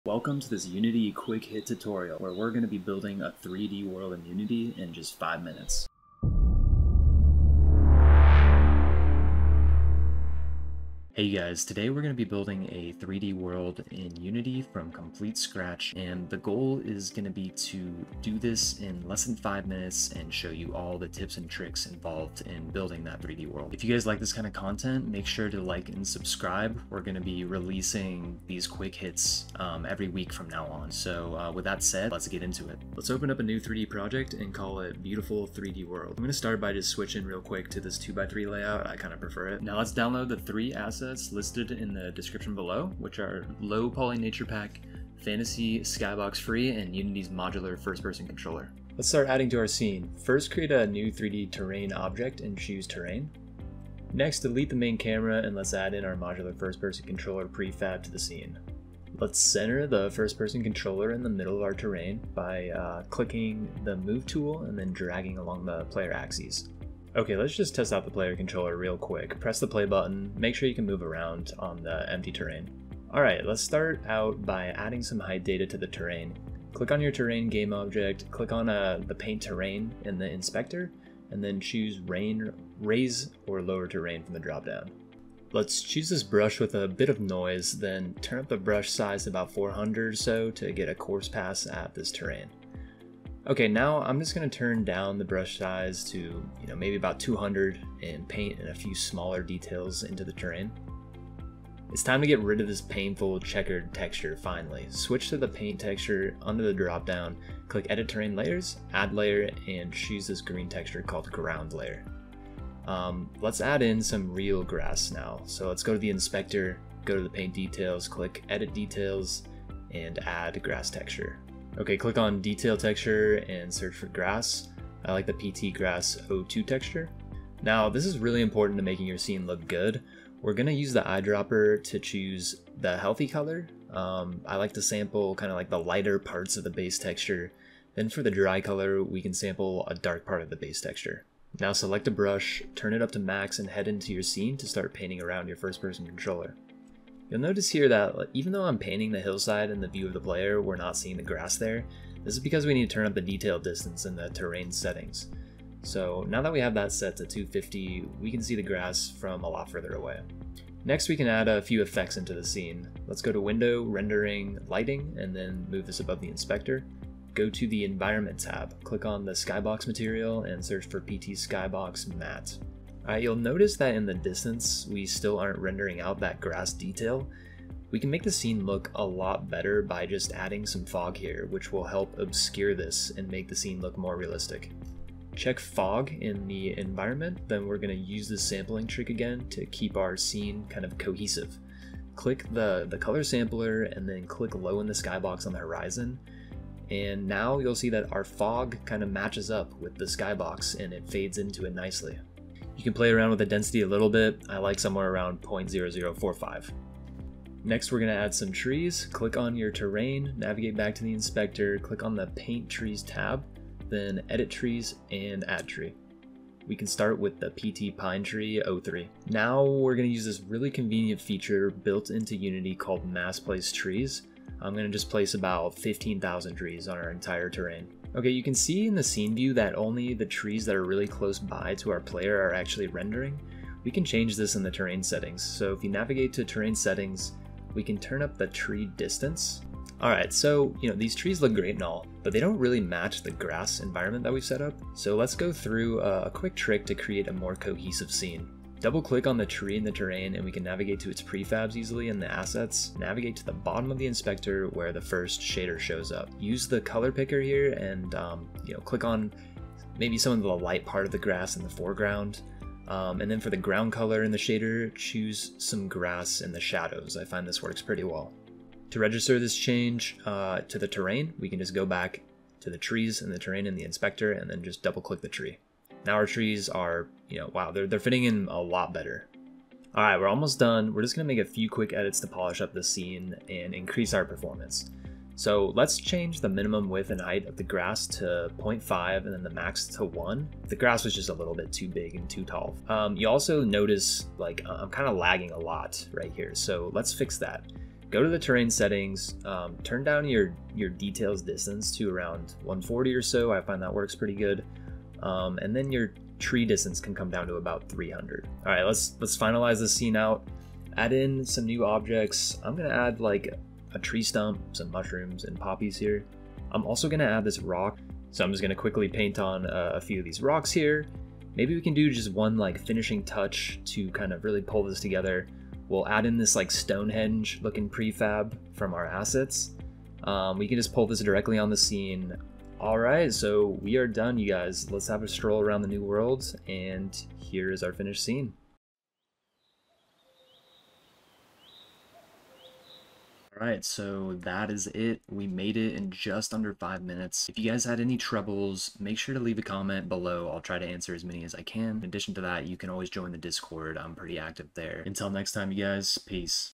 Build a beautiful 3D open world in 5 minutes | Unity
Let's make a beautiful open world in just 5 minutes! In this tutorial, we make a 3D world in Unity from scratch using the built-in terrain system.

Chapters:

0:00 Intro
0:33 Terrain setup
1:40 Textures and grass
2:40 Lighting and effects
3:27 Trees and objects
5:32 Final project
5:42 Outro